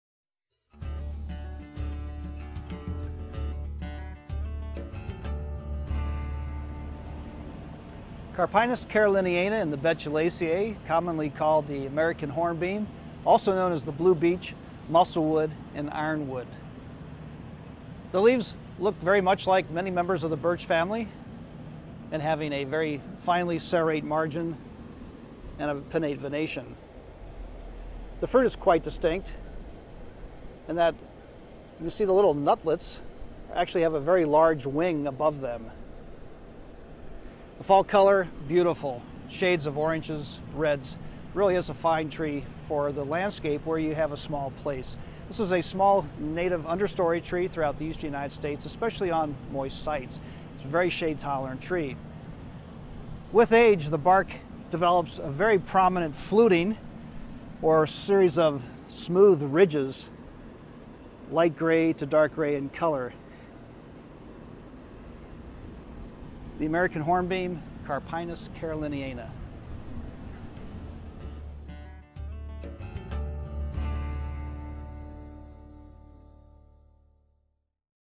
Trees with Don Leopold - American hornbeam
Professor Don Leopold demonstrates the characteristics of American hornbeam.

Content produced by Christopher Baycura for the SUNY College of Environmental Science and Forestry (SUNY-ESF).